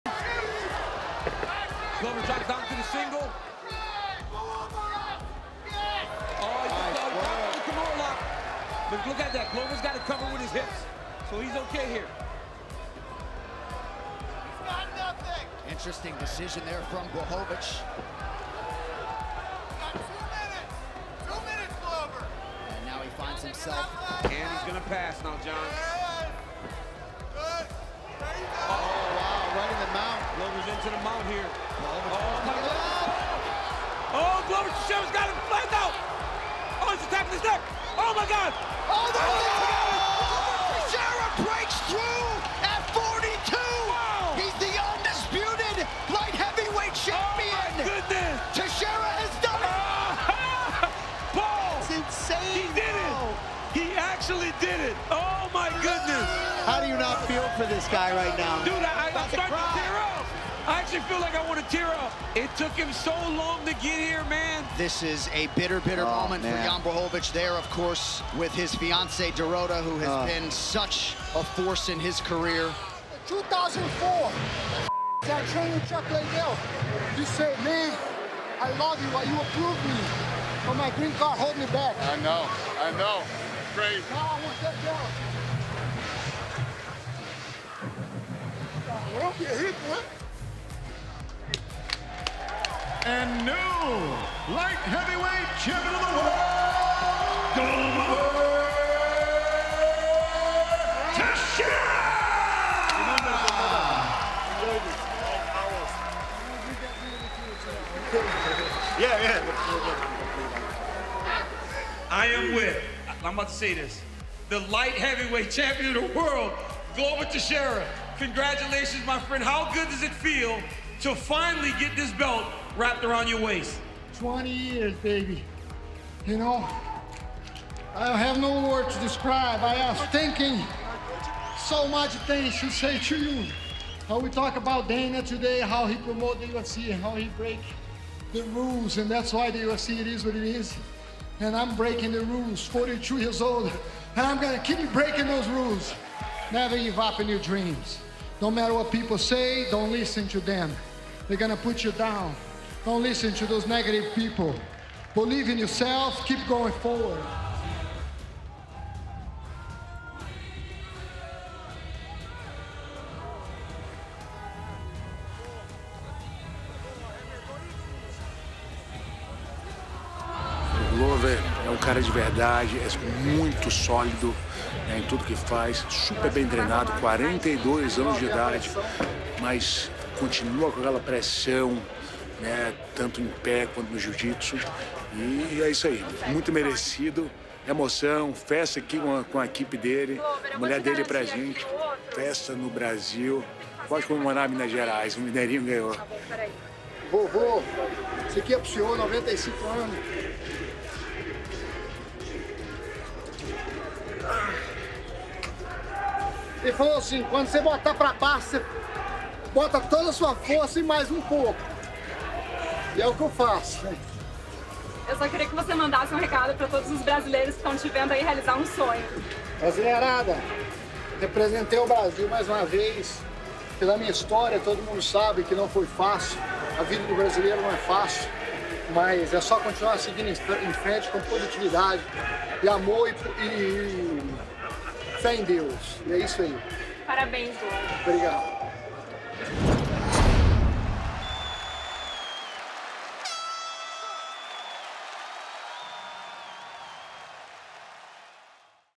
UP CLOSE: Glover Teixeira Climbs the Mountain to Claim UFC Gold at 42 Years of Age
Get an up-close look at Glover Teixeira's light heavyweight championship fight with Jan Blachowicz at UFC 267 last year in Abu Dhabi. The newly crowned champ will be taking on Jiří Procházka at UFC 275 in June.